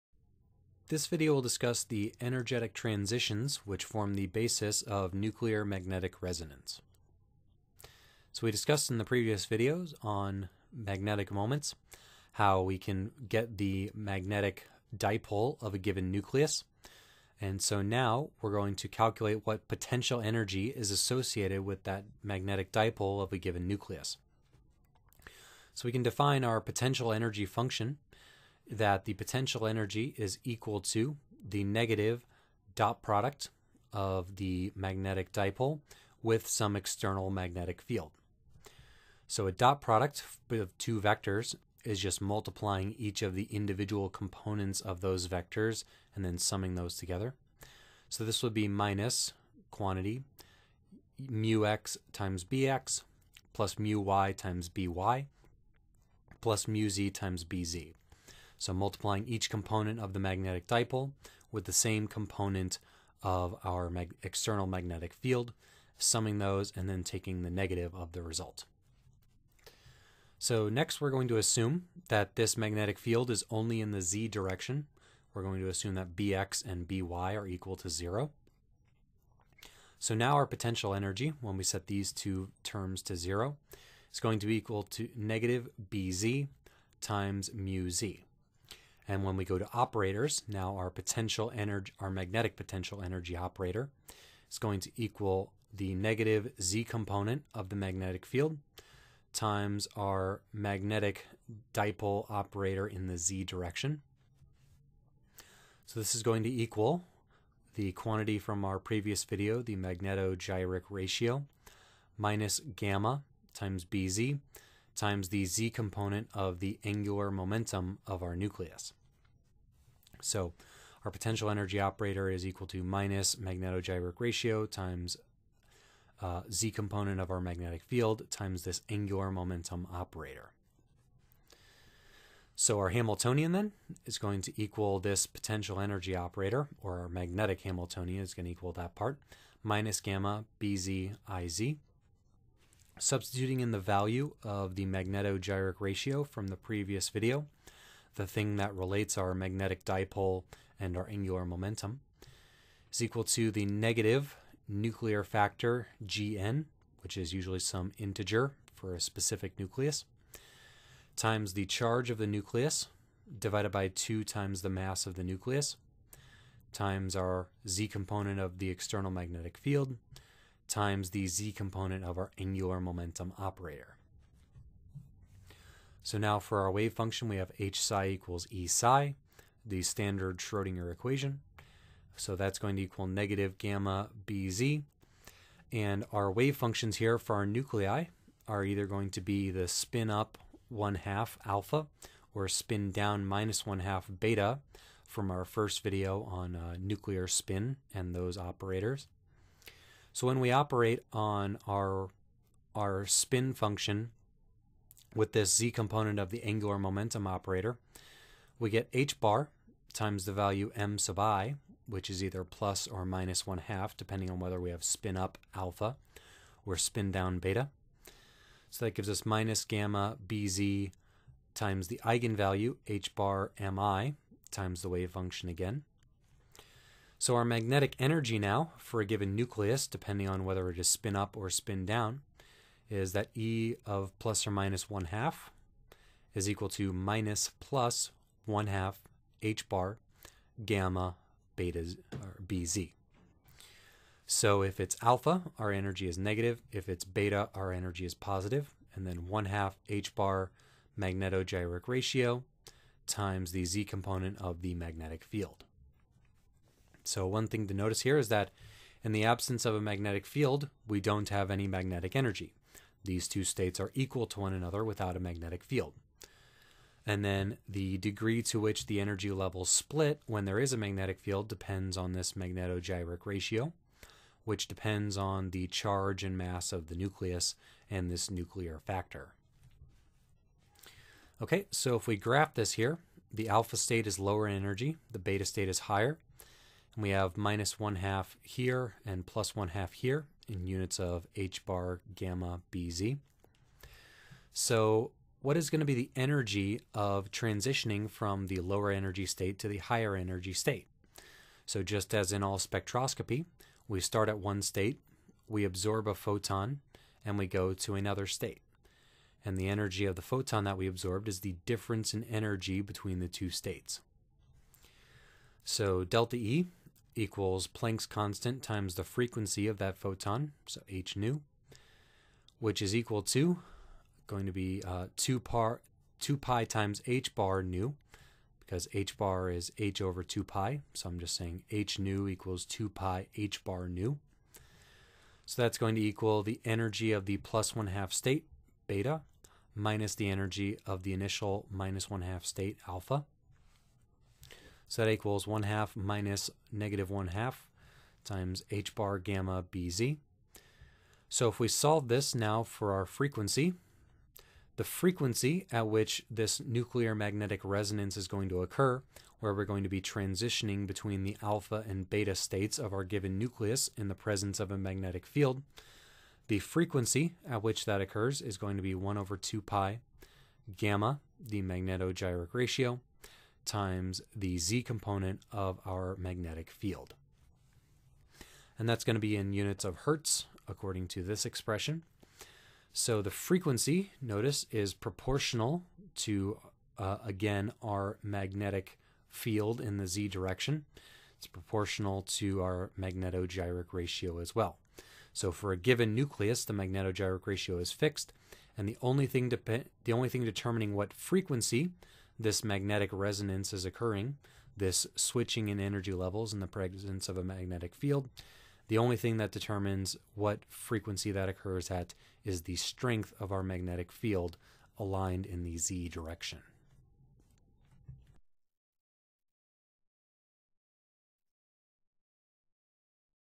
Quantum Chemistry 13.3 - Nuclear Magnetic Resonance
Short lecture on nuclear magnetic resonance.

The magnetic dipoles of spin 1/2 nuclei interact with a magnetic field so generate two states of different total energy. The frequency of photon absorbed to transition between these two energy levels is determined by the strength of the magnetic field and the magnetogyric ratio of the nucleus.

--- Video Links ---

--- Social Links ---

--- Equipment ---

Drawing Tablet: Wacom Intuos Pen and Touch Small

Drawing Program: Autodesk Sketchbook Express